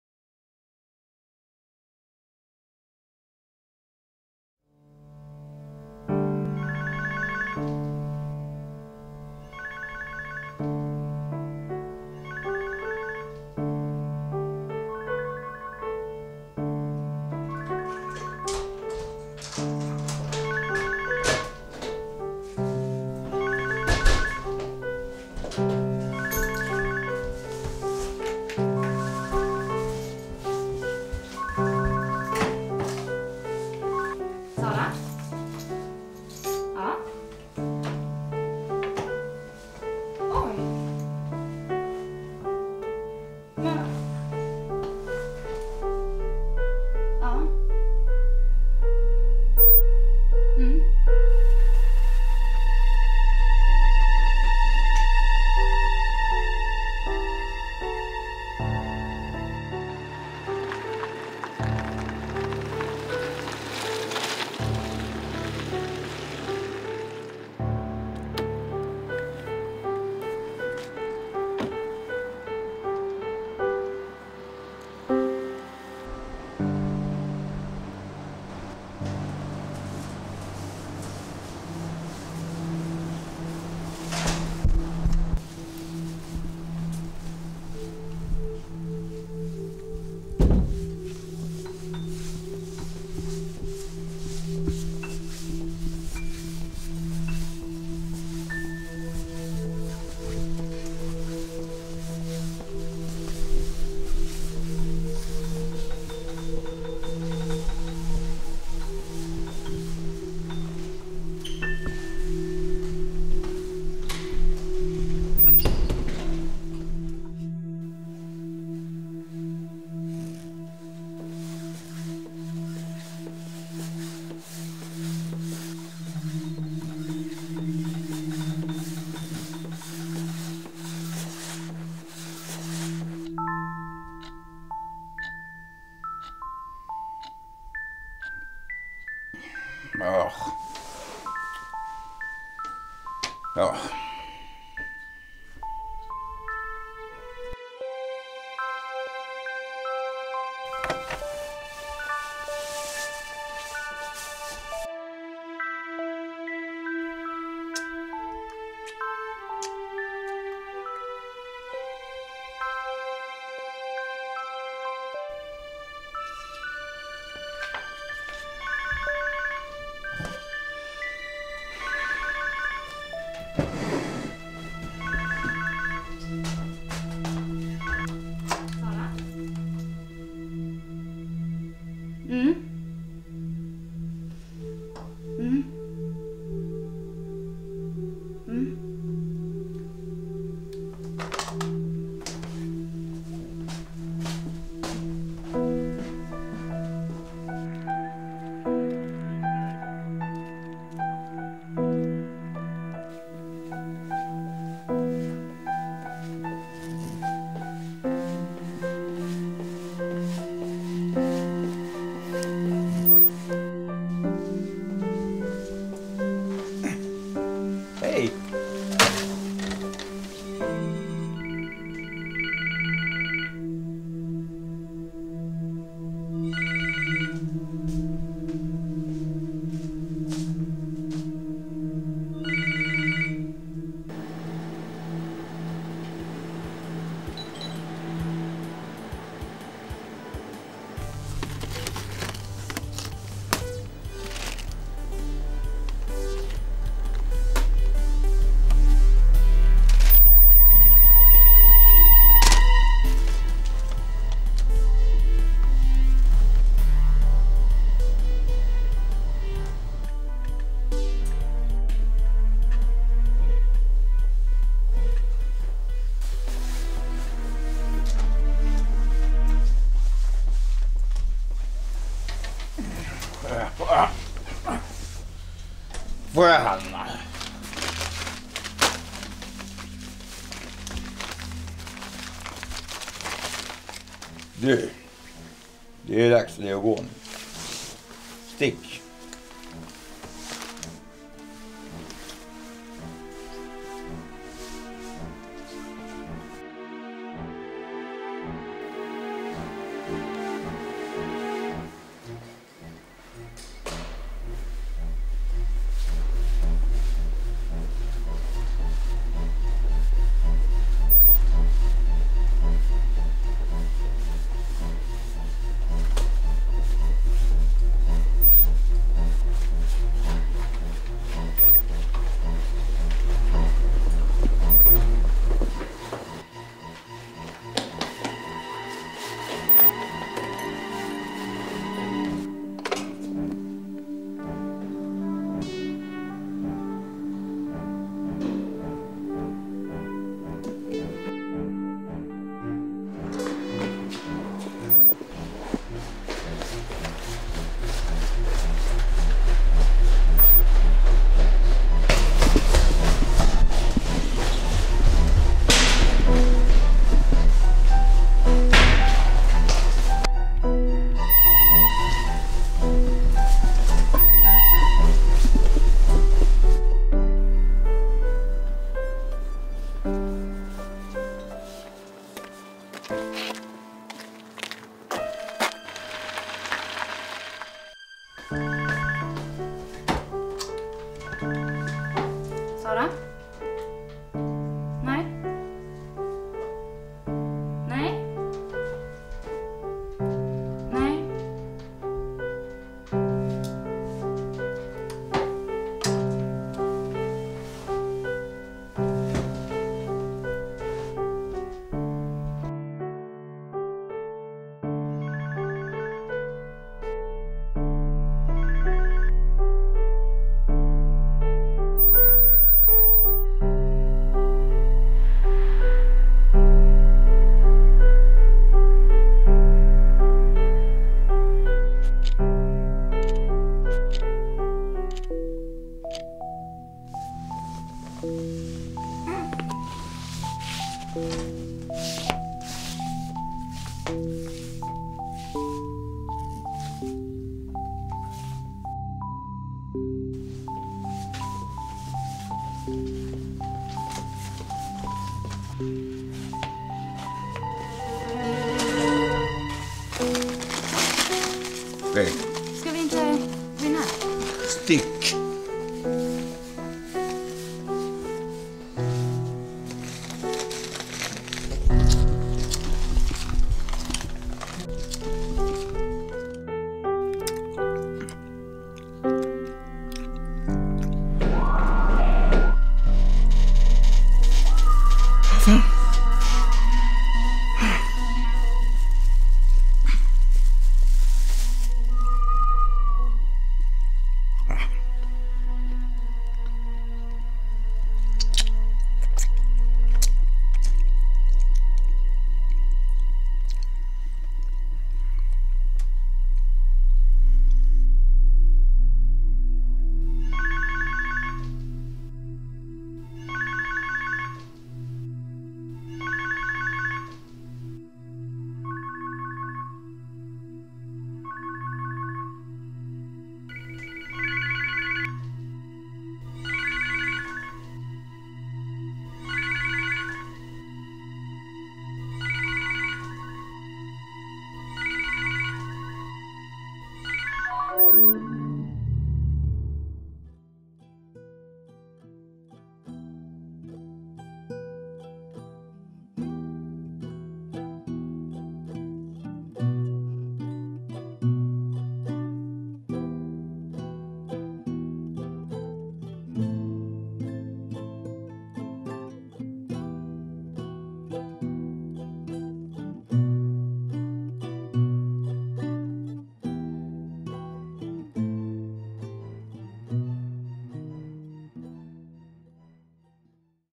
Korridoren
Någonting verkar finnas där. Kan något hända henne? Hon vill inte dit men tvingas! Korridoren känns jobbig! Filmen handlar om det som finns eller inte finns i ögonvrån bakom oss! Sara spelas av Micaela Karlsson och mannen i sängen av Bosse Emanuelsson.